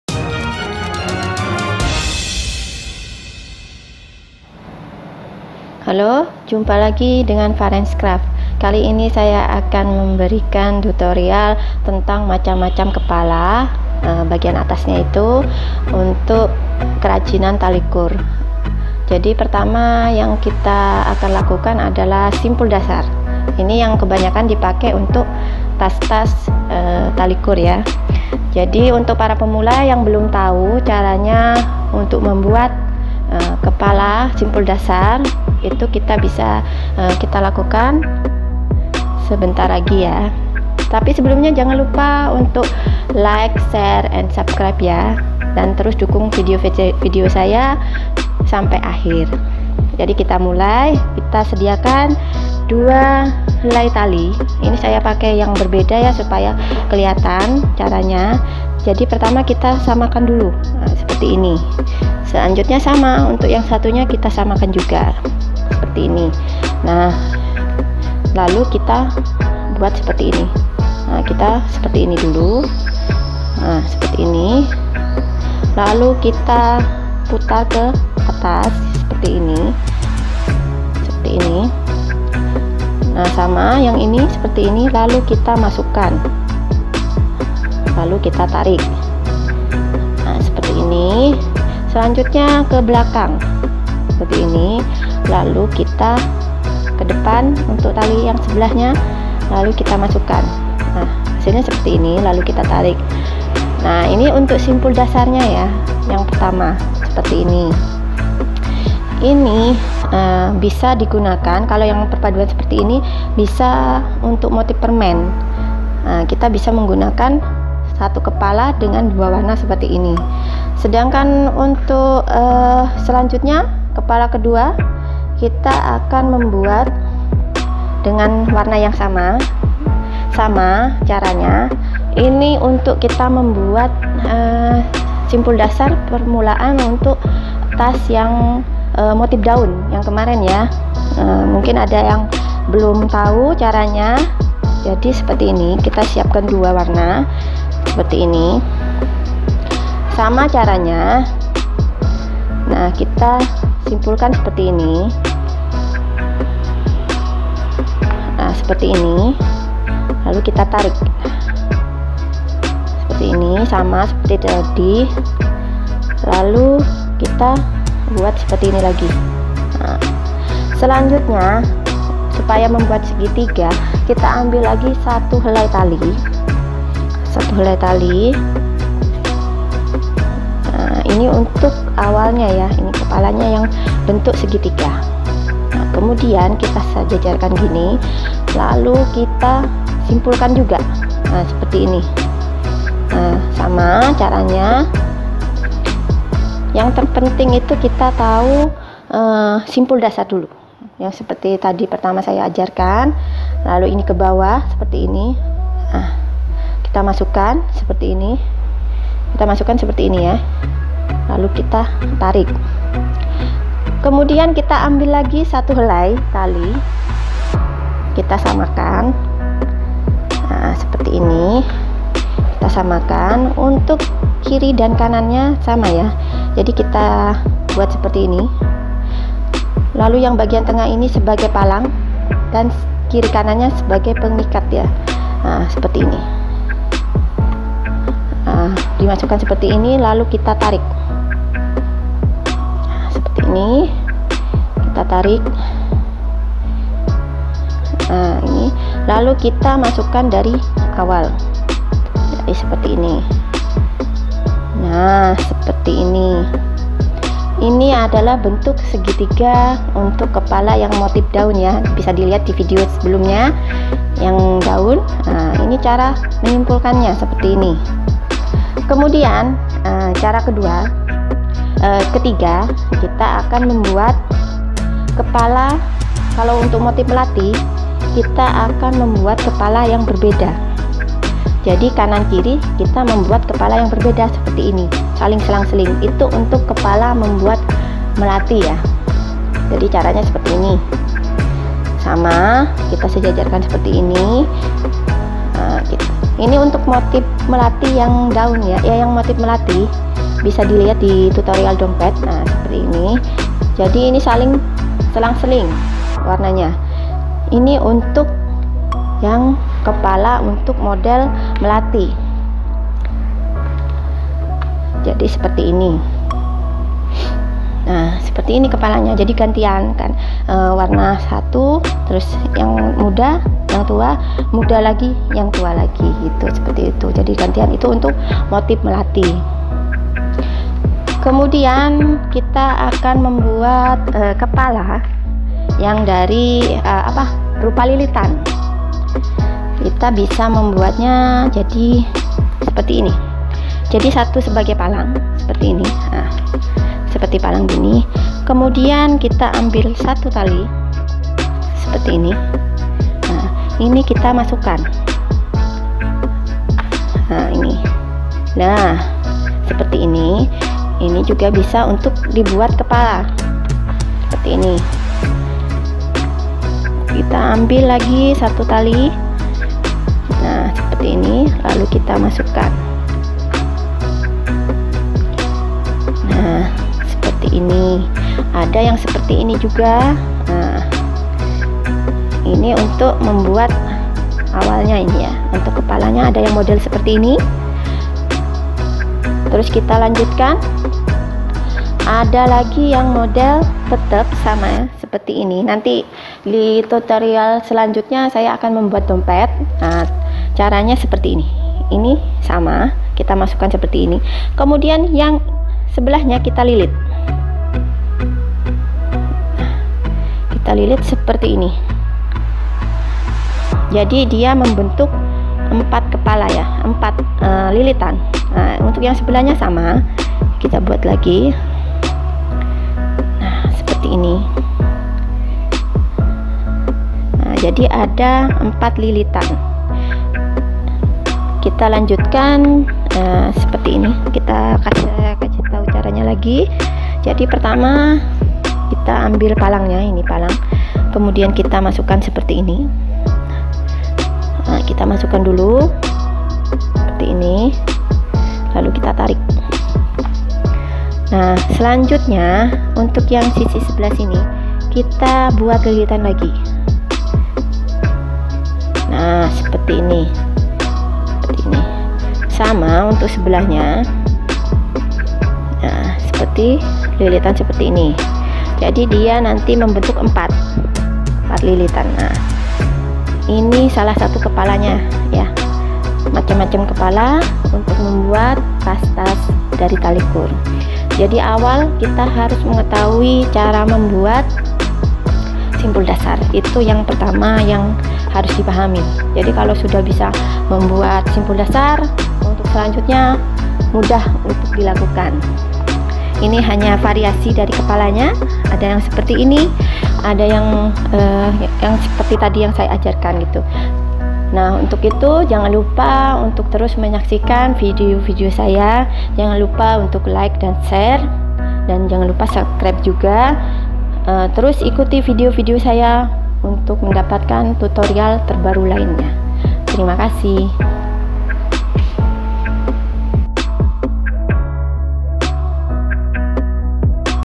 Simpul dasar atau cara membuat kepala tas tali kur, pemula wajib tahu.
Basic macrame knot for beginner, simpul simpul dasar tali kur

Video ini tentang bagaimana membuat simpul dasar tali kur yang biasa dipakai pada kerajinan tali kur seperti : tas tali kur, dompet tali kur dan lain lain.